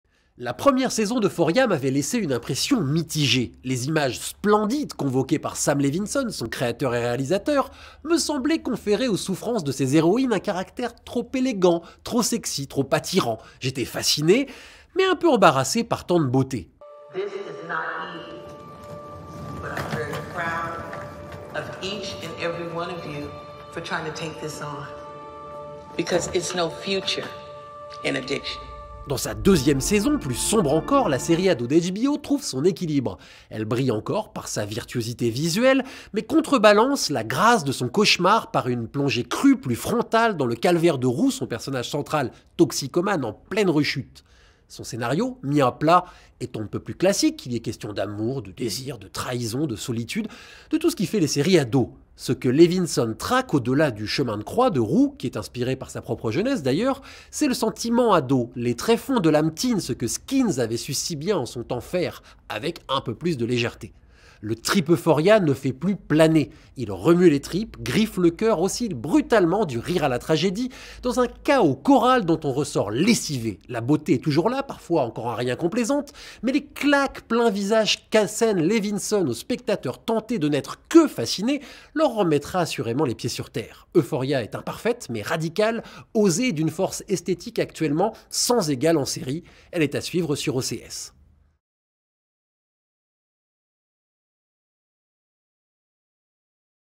La Reco séries de Pierre Langlais : Euphoria - Le Cercle Séries
« Euphoria est imparfaite, mais radicale. C'est une série osée, d’une force esthétique actuellement sans égale en série. »

Pierre Langlais recommande fortement Euphoria sur OCS, disponible avec CANAL+